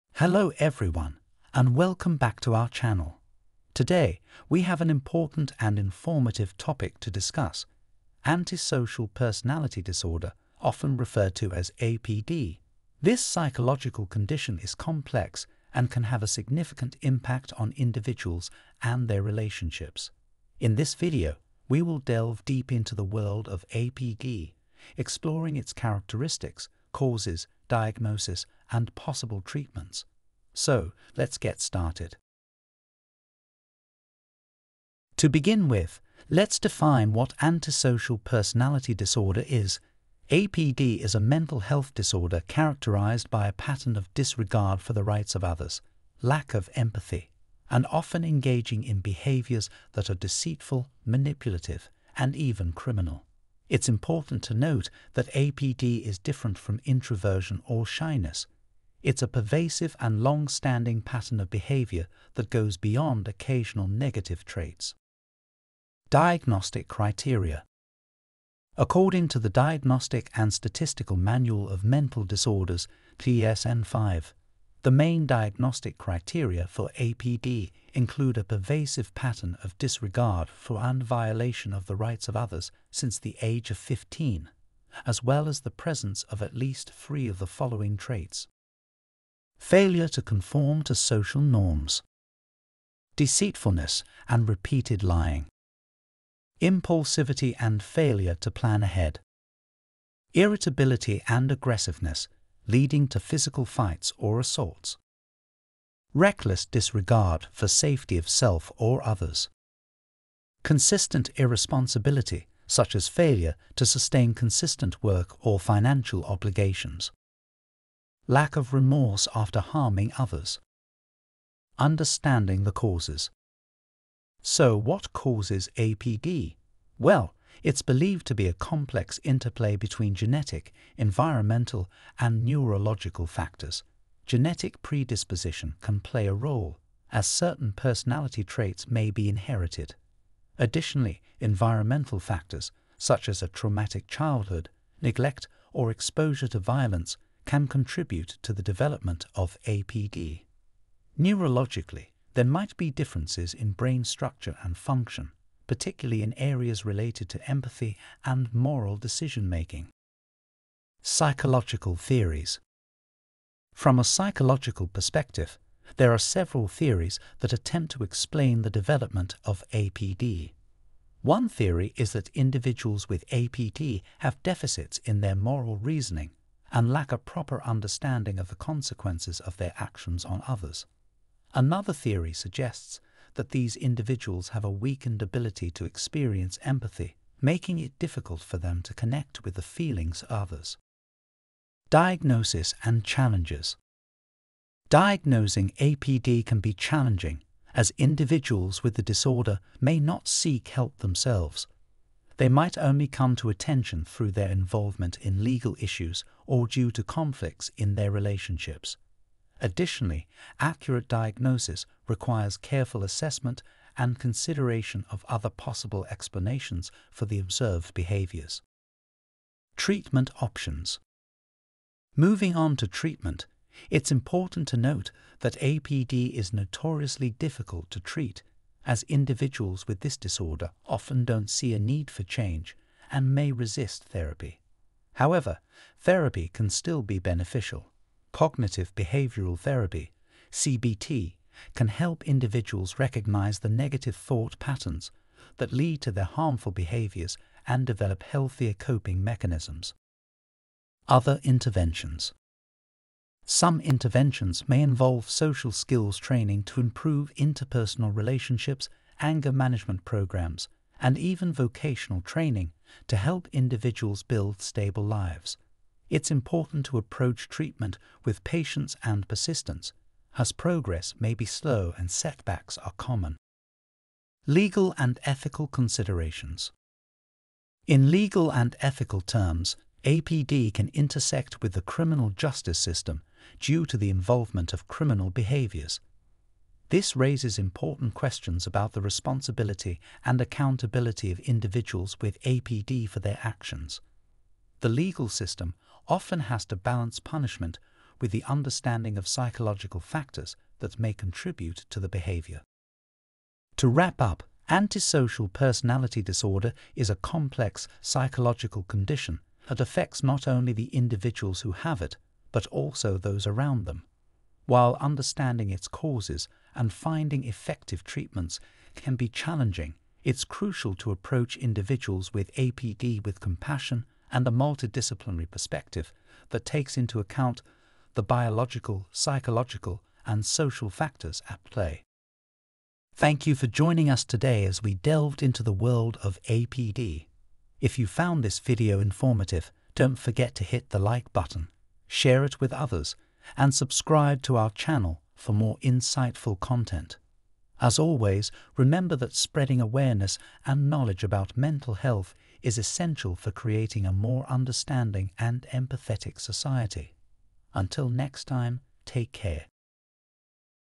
Understanding Antisocial Personality Disorder (APD) from Psychological Perspective: DSM 5, treatment
In this informative video, we delve deep into the world of psychology to help you understand Antisocial Personality Disorder (APD) from a psychological perspective. APD is a complex mental health condition characterized by a pattern of disregard for the rights of others, lack of empathy, and often engaging in deceitful and manipulative behaviors.

Join us as we explore the defining traits of APD, the challenges in diagnosis, potential causes including genetic, environmental, and neurological factors, and the psychological theories that shed light on its development. We'll also discuss treatment options, legal considerations, and the impact of APD on relationships and society.

By increasing awareness and knowledge about APD, we aim to foster understanding and empathy for individuals who may be dealing with this condition.